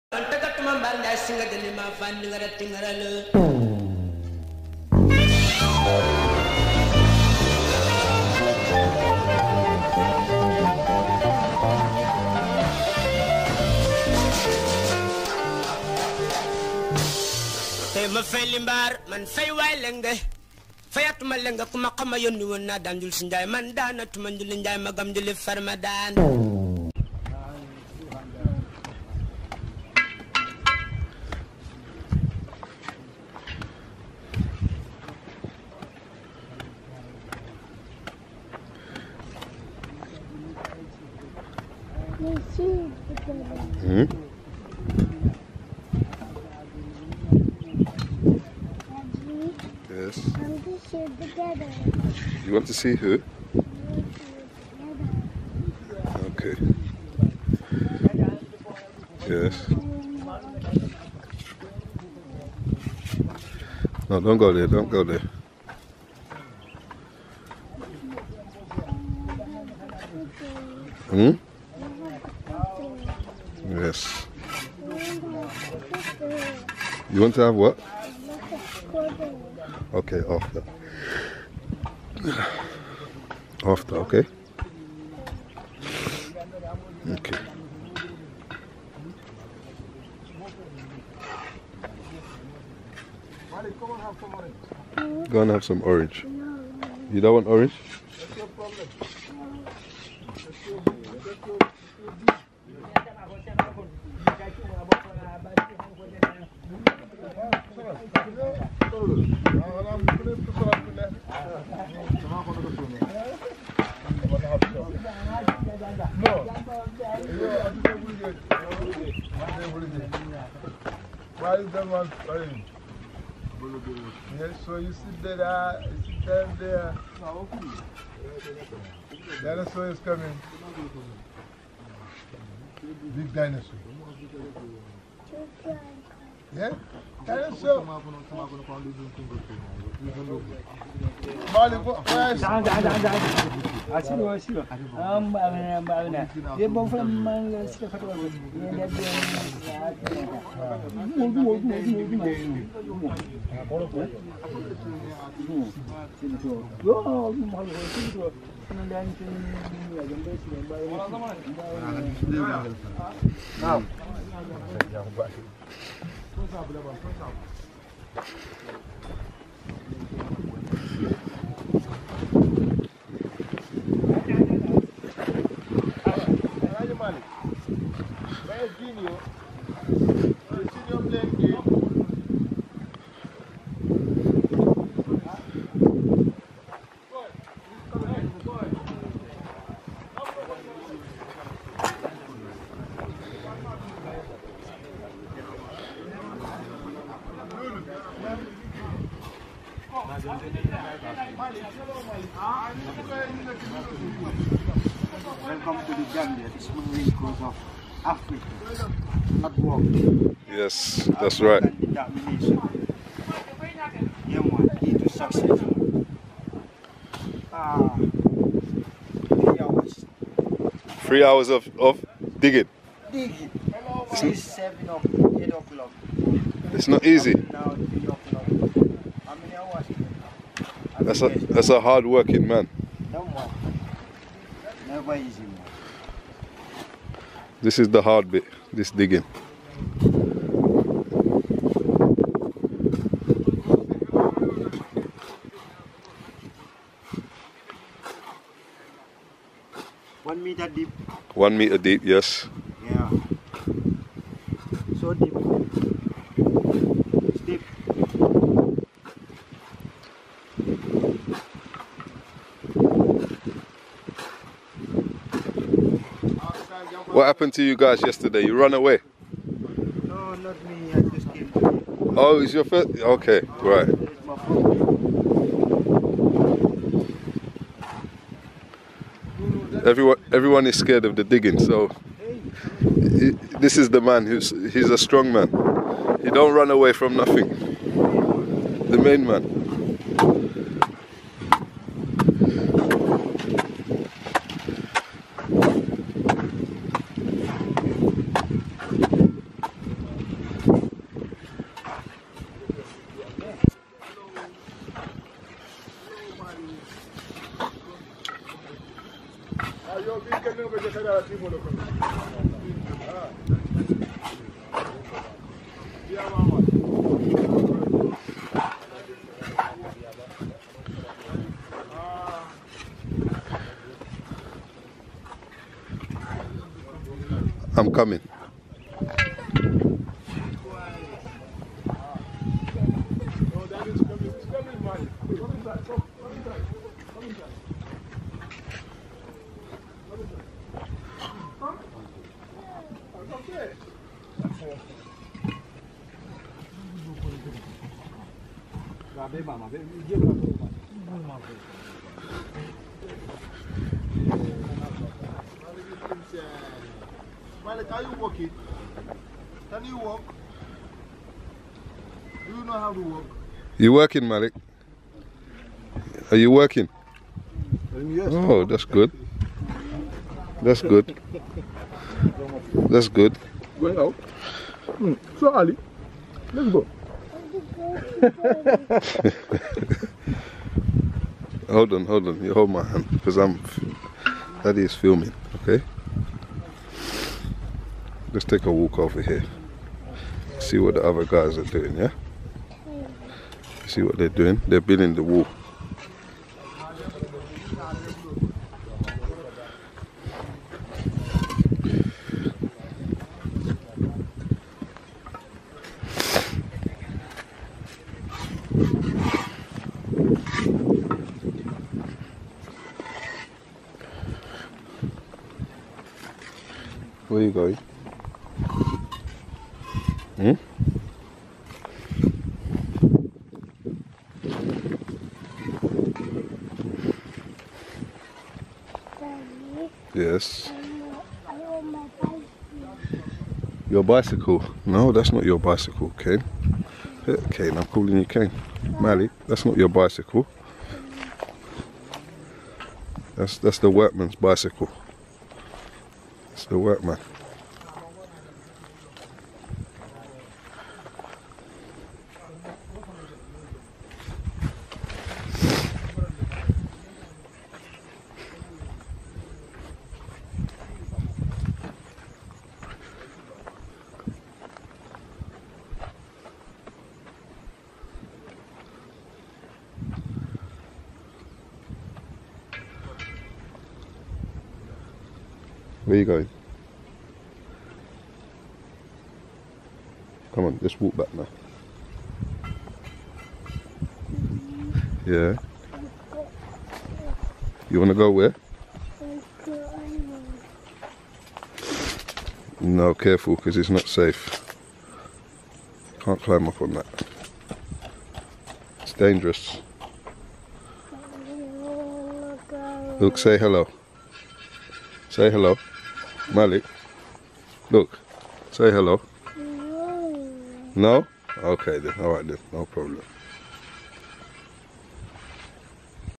(Building a house in Gambia Africa) continuation of the house foundation. Part 5
Showing you step by step, for the foundation of the house. Digging the ground, and laying the concrete, blocks, and metal work.